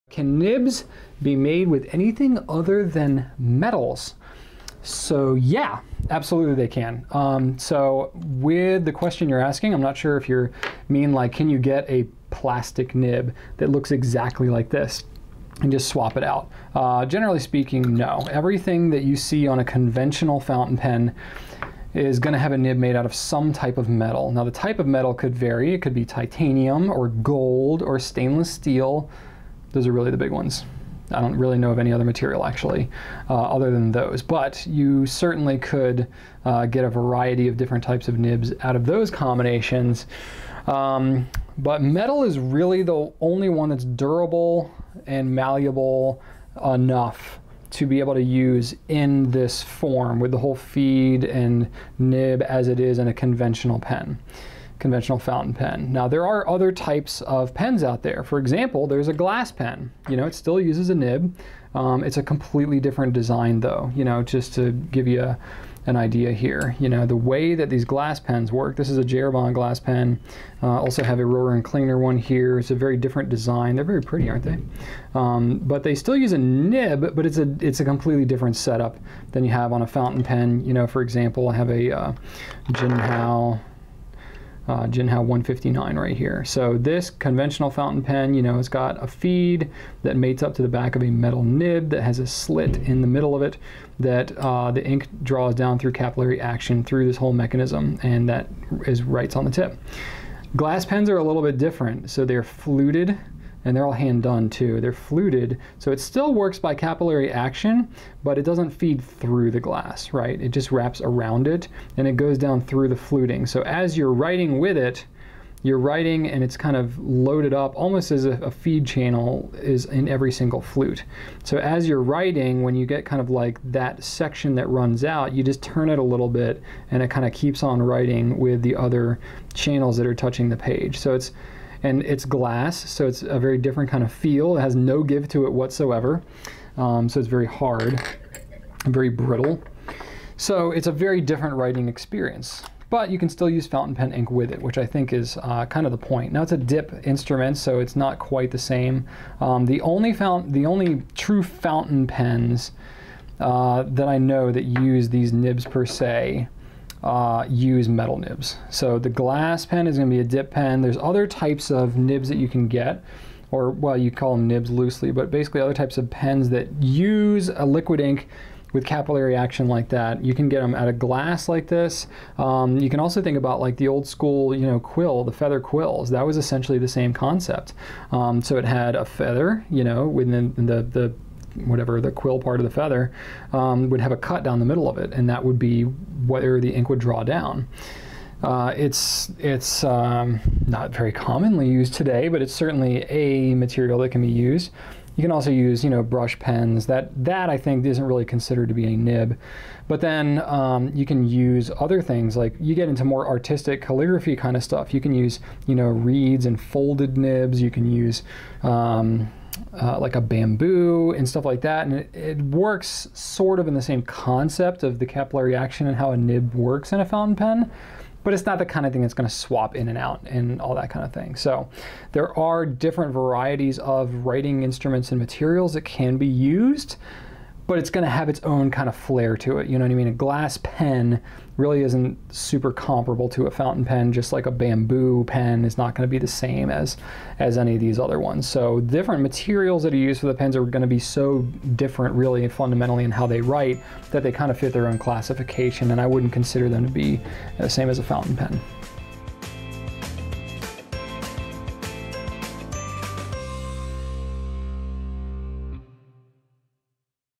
Can Nibs Be Made Of Anything Other Than Metal? - Q&A Slices
Can nibs be made with anything other than metals?

CAN they? Sure, but not often are they
metal is durable and malleable enough to work with
There are glass pens
There are wood/bamboo/reed pens (that artists use)
feathers (quills)
really any natural material can be a nib, it'll just look really different than metal ones